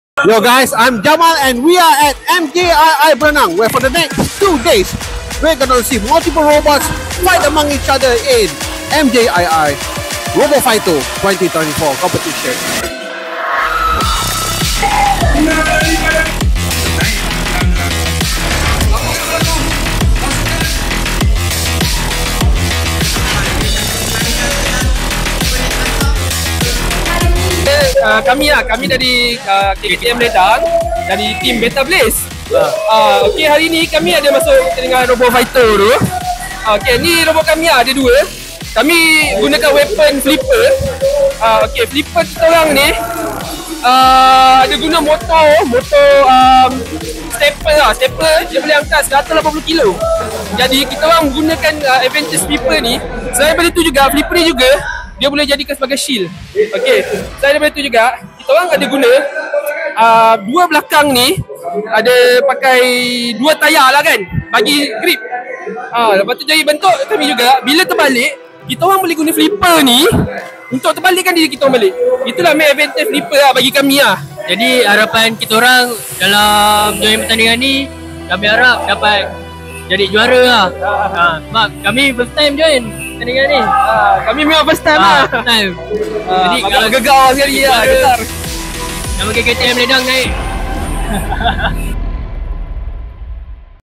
𝗣𝗘𝗥𝗧𝗔𝗥𝗨𝗡𝗚𝗔𝗡 𝗕𝗘𝗥𝗠𝗨𝗟𝗔! | 𝗠𝗝𝗜𝗜 𝗥𝗢𝗕𝗢𝗙𝗜𝗚𝗛𝗧𝗢 𝗖𝗢𝗠𝗣𝗘𝗧𝗜𝗧𝗜𝗢𝗡 𝟮𝟬𝟮𝟰
Jangan lepaskan peluang pada 19 - 20 Oktober 2024 untuk menyaksikan robot-robot tercanggih bertarung hebat dalam Pertandingan MJII Robofighto 2024 yang diadakan di MARA Japan Industrial Institute (MJII), Beranang. Sejak 2016, ini adalah platform yang dinantikan oleh semua peminat inovasi, teknologi, dan kreativiti dalam dunia robotik.

Siapakah yang bakal menjuarai pertempuran tahun ini? Saksikan kecerdikan dan strategi dalam menguasai gelanggang!

Tonton sekarang di FB Live!